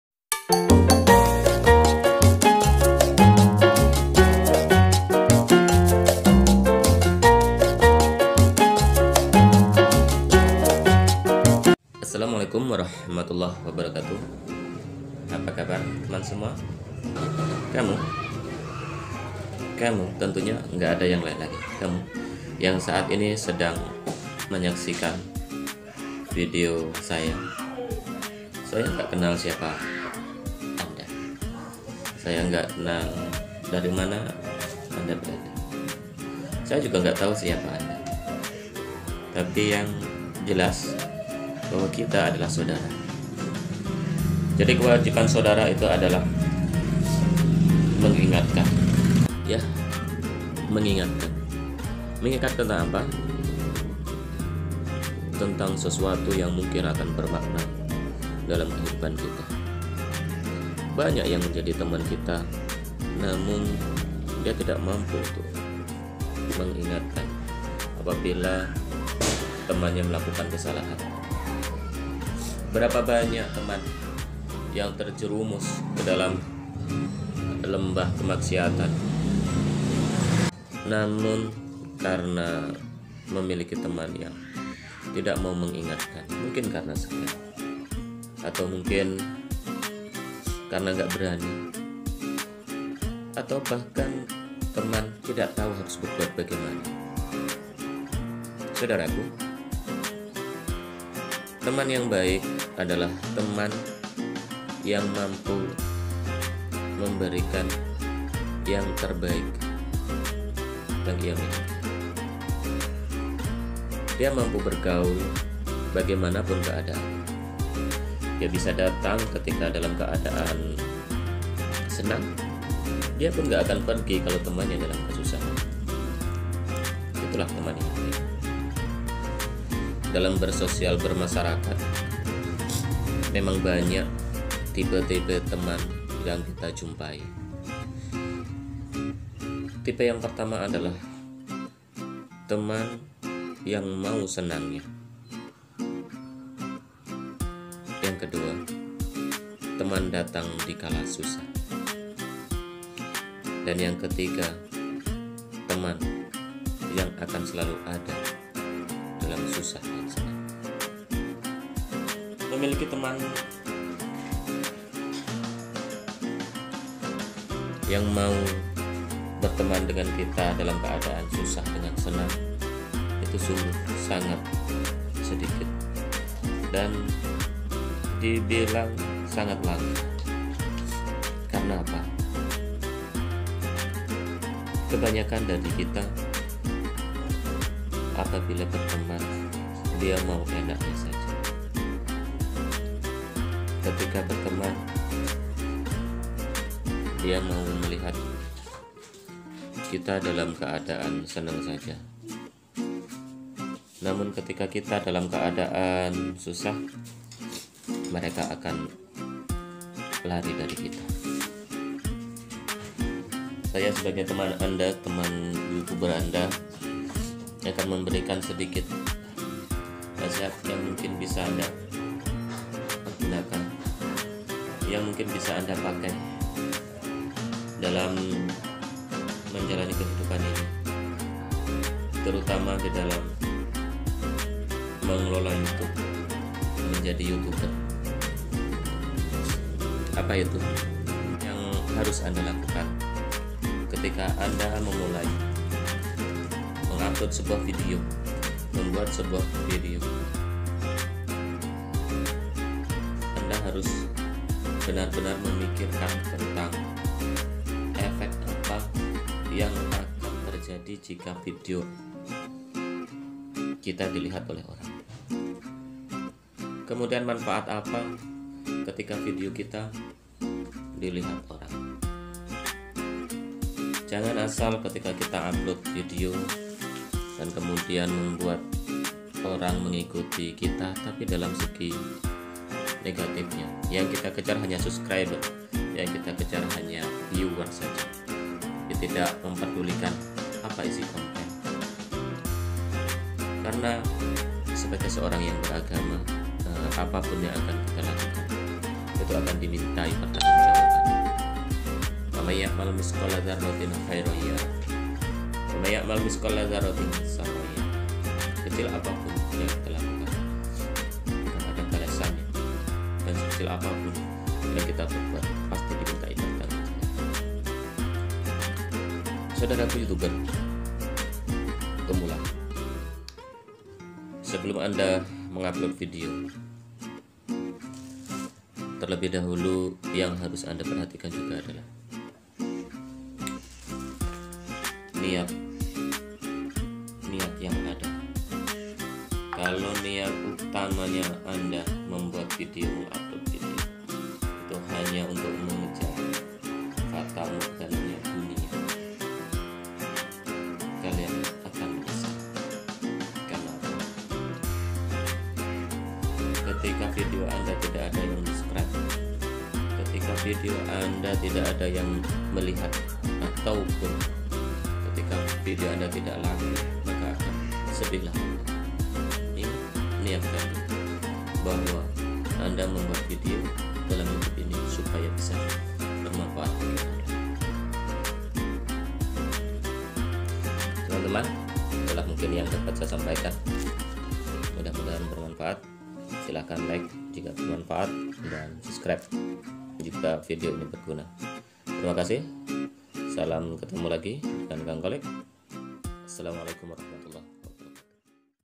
Untuk mu youtuber pemula
Perhatikan sebelum membuat video untuk apa dan apa manfaat nya.